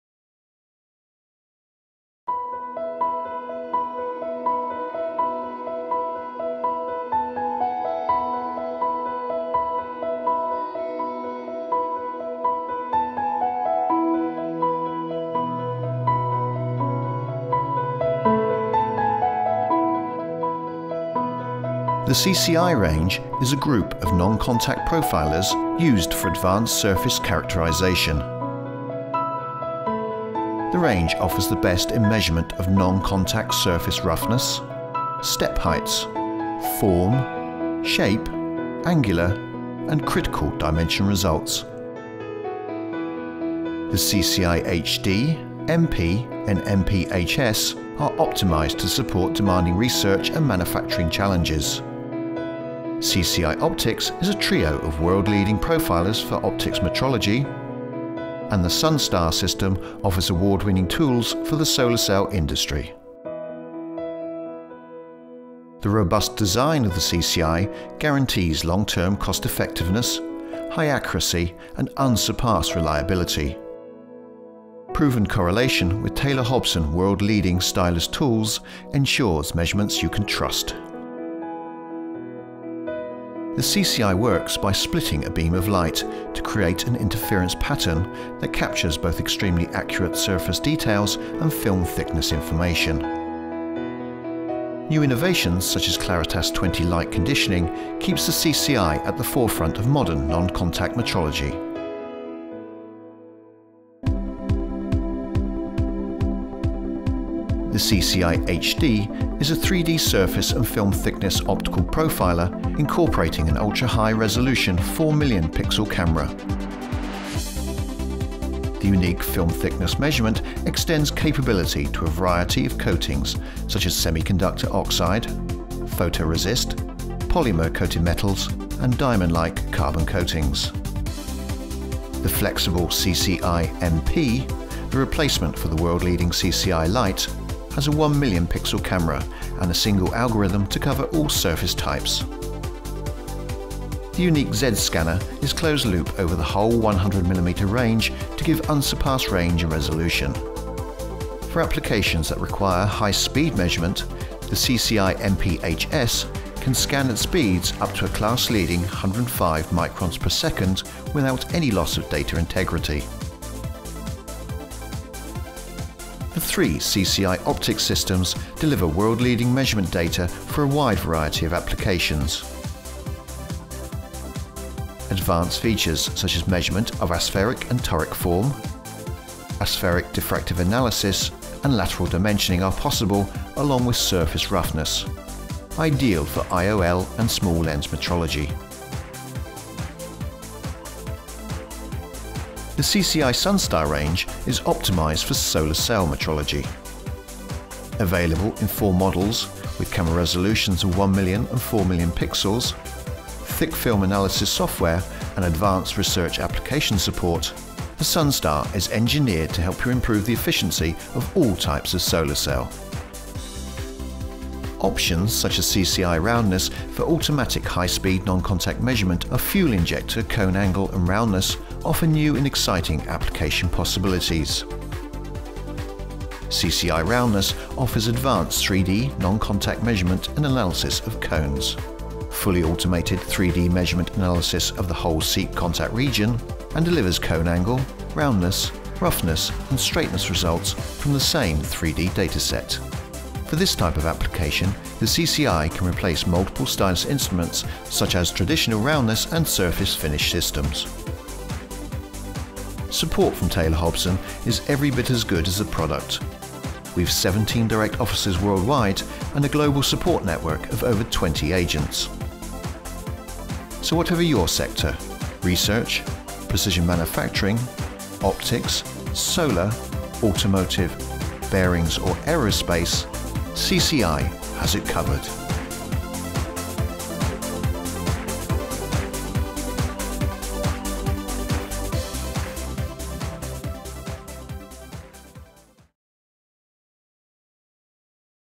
Taylor-Hobson - Strumenti della serie CCI
Continua l’impegno di Taylor-Hobson a fornire soluzioni di massima precisione, risoluzione e affidabilità. L’azienda presenta tutte le qualità degli strumenti della serie CCI in un video.